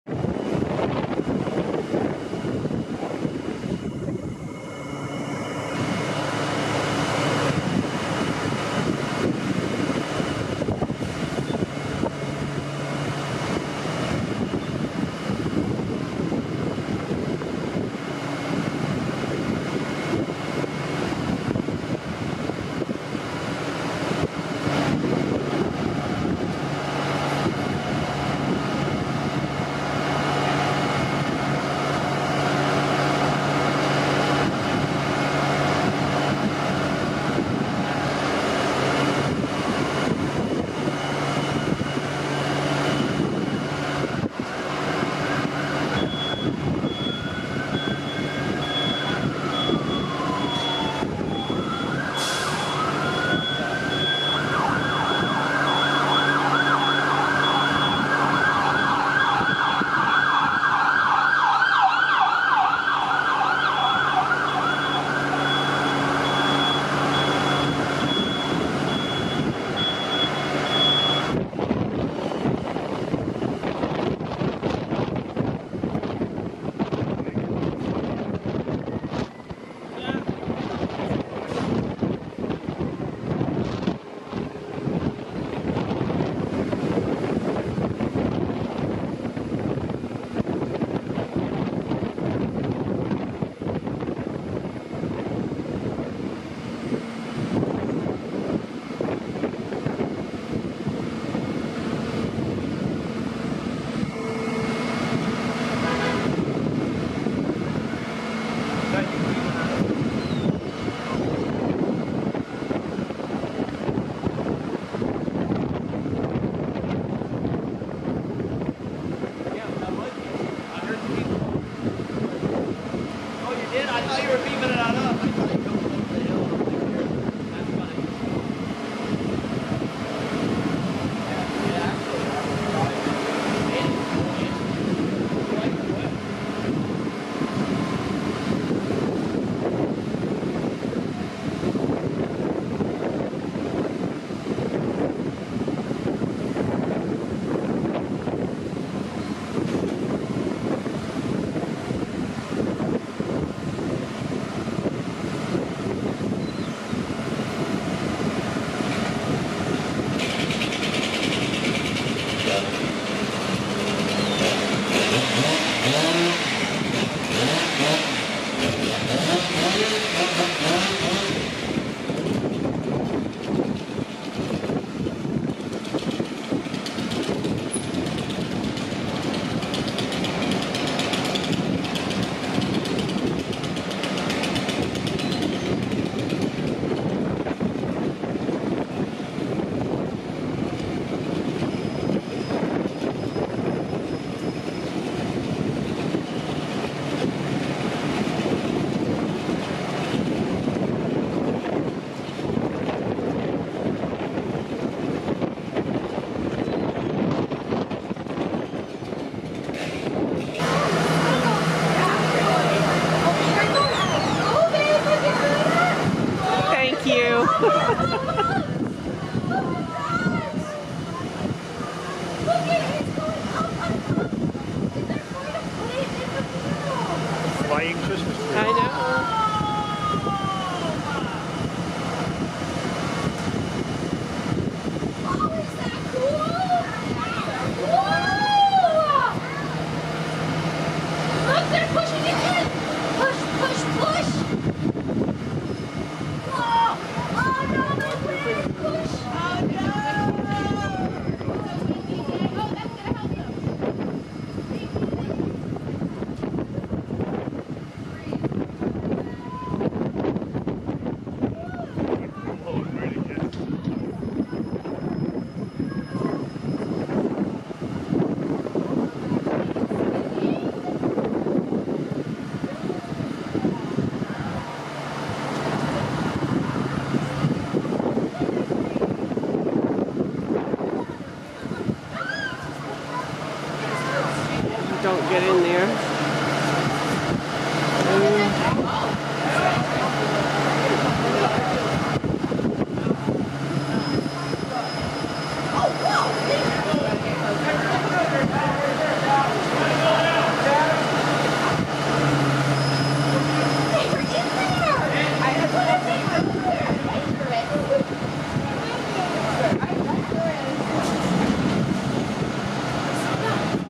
Burlington Church Street Tree Raising 2023
From Church Street Marketplace:

Burlington’s holiday tree, which stands at the top of the Church Street Marketplace, is delivered on Wednesday, November 15th.

11/15/2023